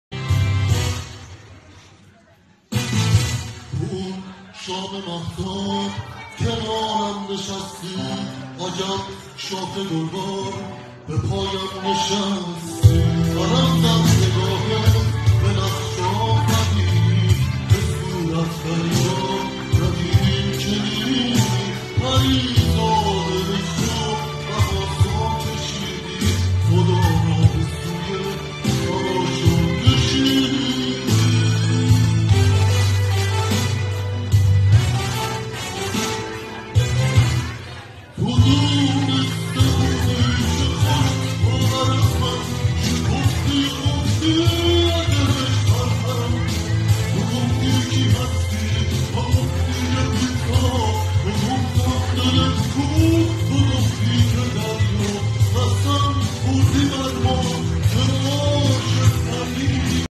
تقلید صدای داریوش
تقلید صدای داریوش توسط منصور جودی